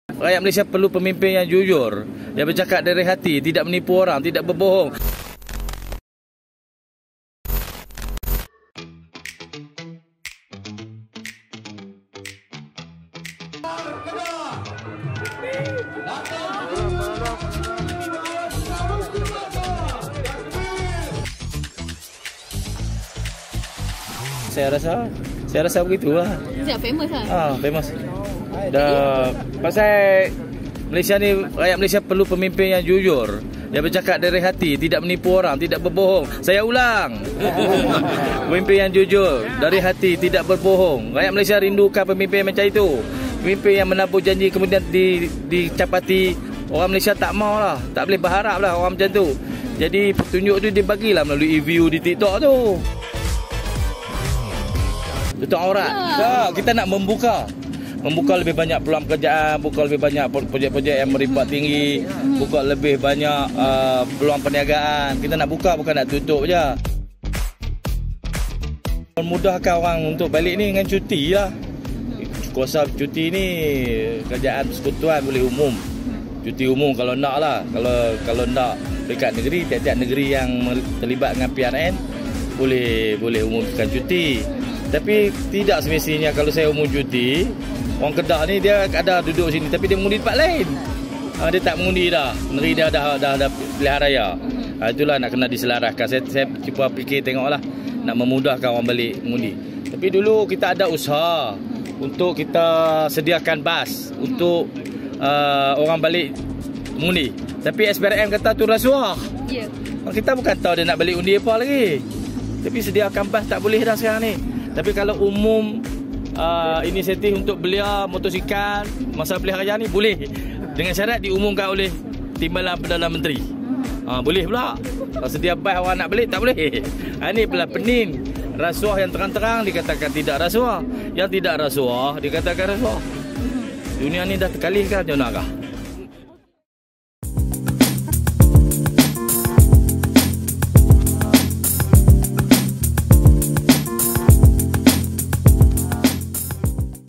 Adakah MB Kedah lebih digemari daripada PMX oleh rakyat Malaysia?
2 Ogos 2023 di Flat Alor Malai Derga alorsetar - YB Muhammad sanusi Md Nor membandingkan dirinya dengan anwaribrahim . Confident level SUPERSTAR